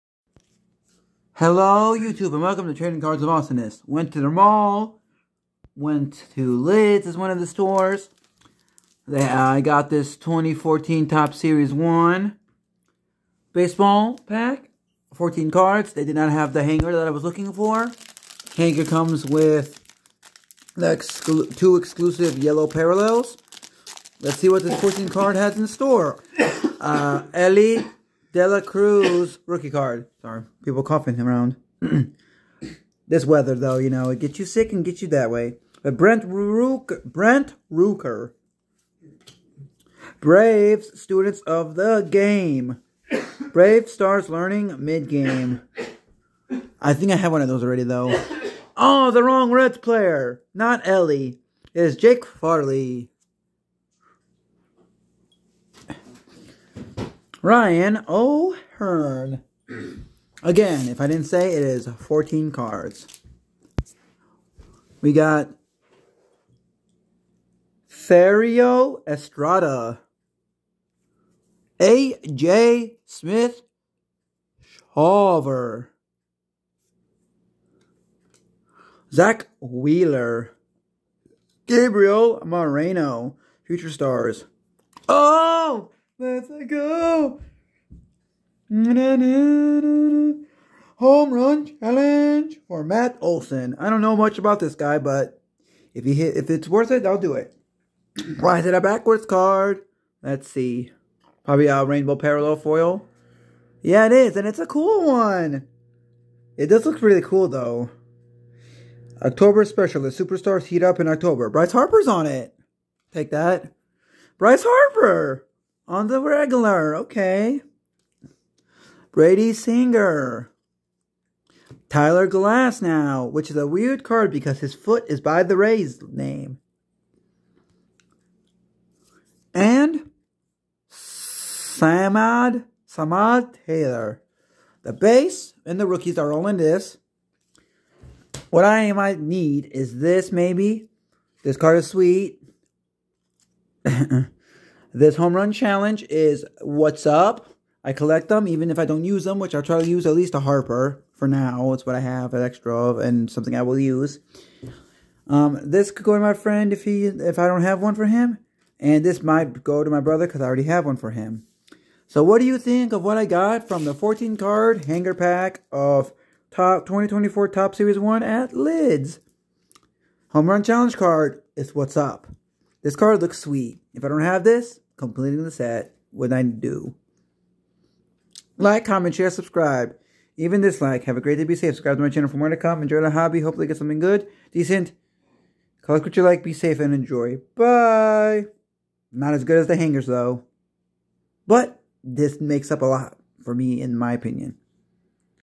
opening 1 2024 Topps Series 1 Pack from Lids
What do you think of what I got? Enjoy!